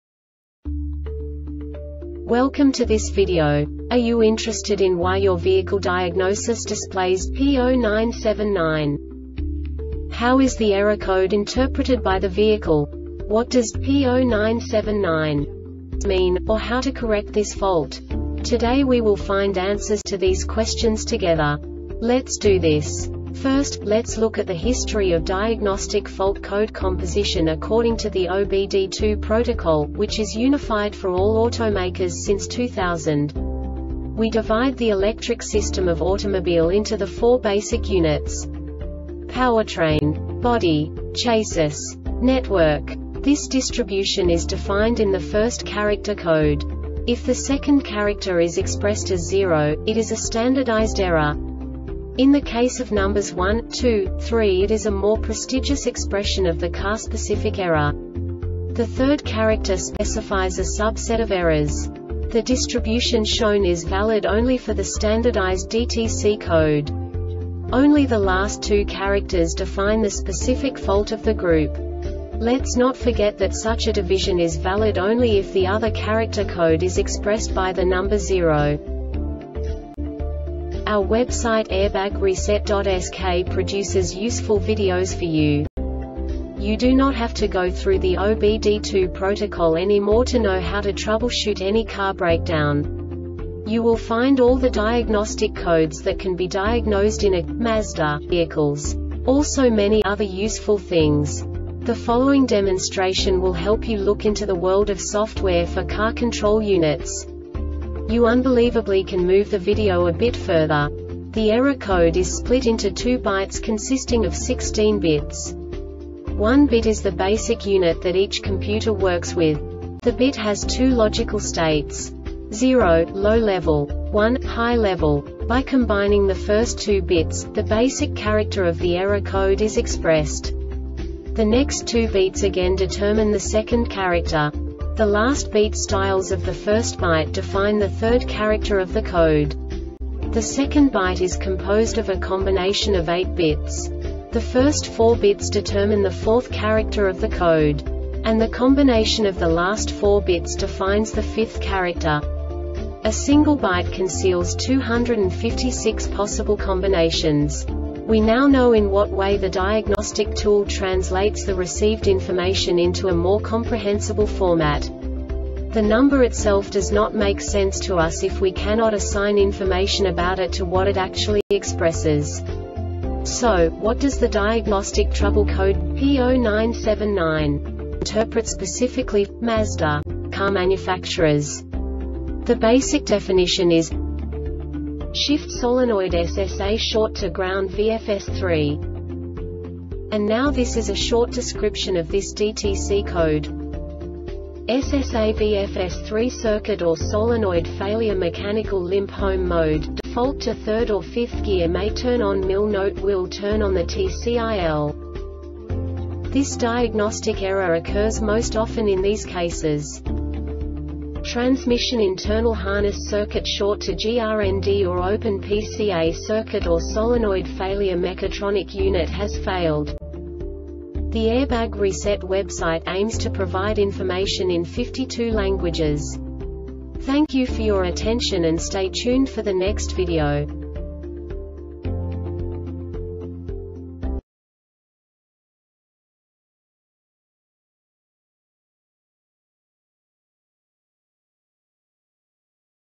DTC Mazda P0979 Short Explanation
Contents:
0:21 Basic DTC analysis according to OBD2 protocol standard.
1:48 Insight into programming
2:58 A diagnostically understandable description of the Diagnostic Trouble Code
3:05 Basic error definition

Make:
Mazda
Code:
P0979
Definition:
Shift Solenoid (SSA) Short To Ground (VFS-3)
Description:
SSA (VFS-3) circuit or solenoid failure.Mechanical limp-home mode,default to 3rd or 5th gear.May turn on MIL.Note: Wil turn on the TCIL.
Cause:
 Transmission internal harness circuit short to GRND or open PCA circuit or solenoid failureMechatronic unit has failed